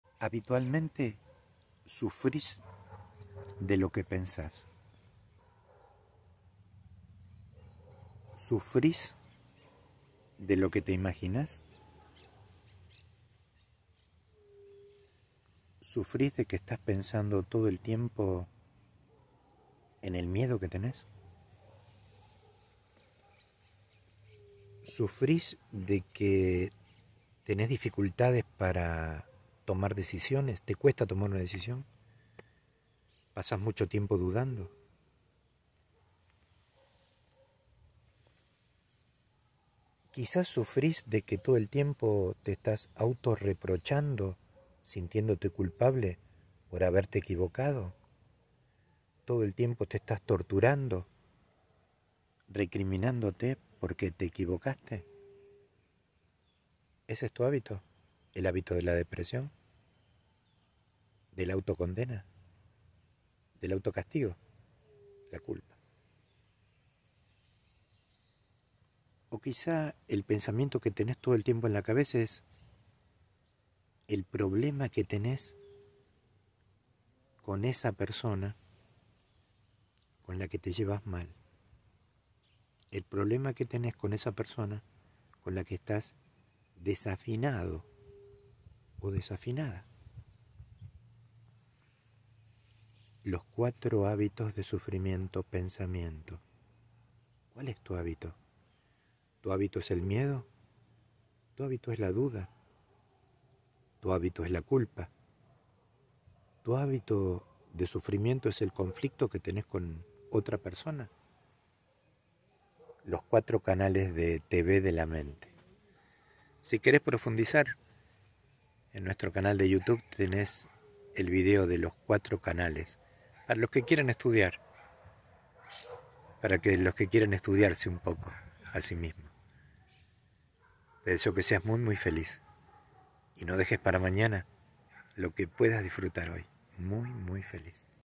tips - Sufris de lo que pensás? - Lic.Alberto Kuselman
Sufris de lo que pensás? Sabias que el 90% de la veces sufrimos de lo que pensamos?
- Comunícate con nosotros para un accesoramiento exclusivo para ayudarte a mejorar tu vida.